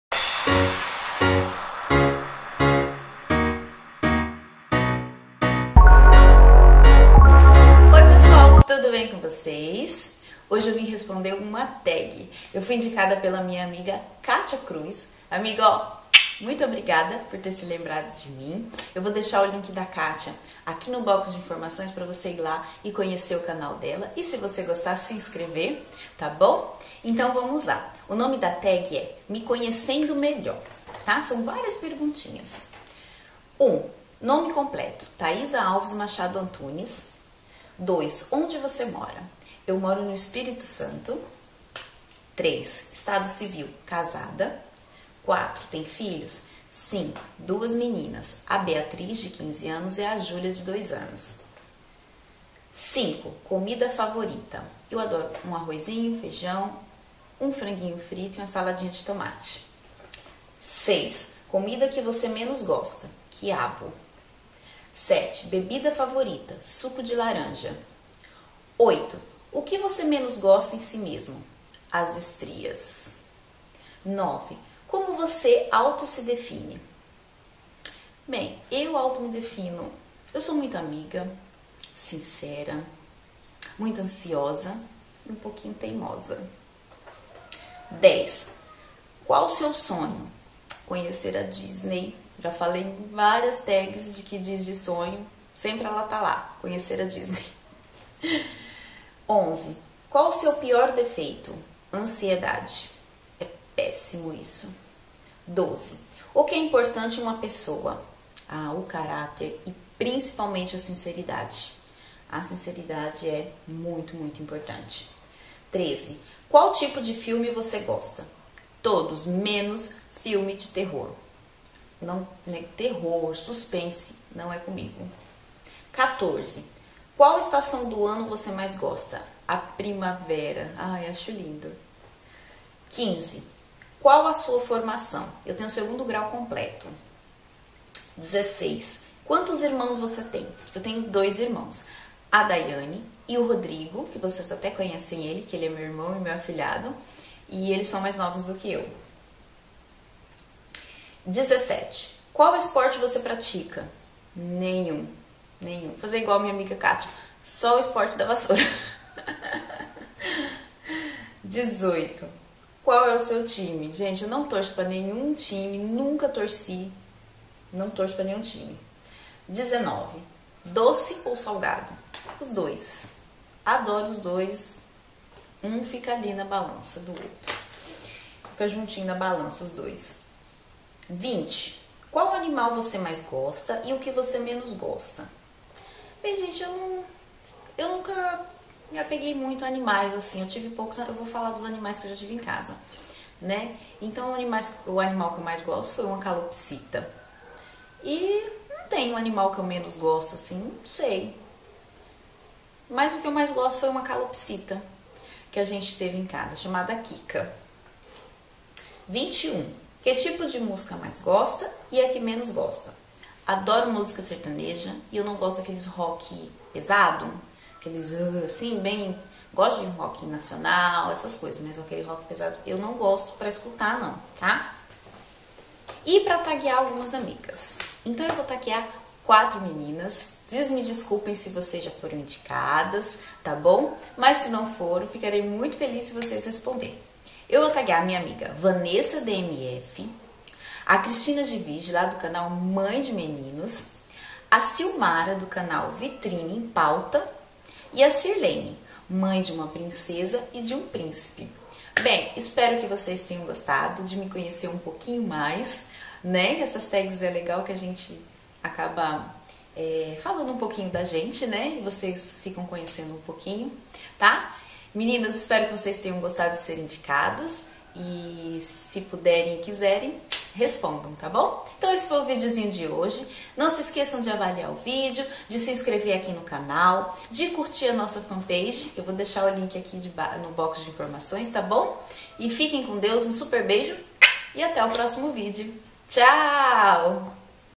Tag - Me Conhecendo Melhor
Hoje vocês irão me conhecer um pouquinho mais nessa tag que irei responder, fui indicada pela Katia Cruz e no final irei indicar quatro meninas. Espero que gostem. bjssssss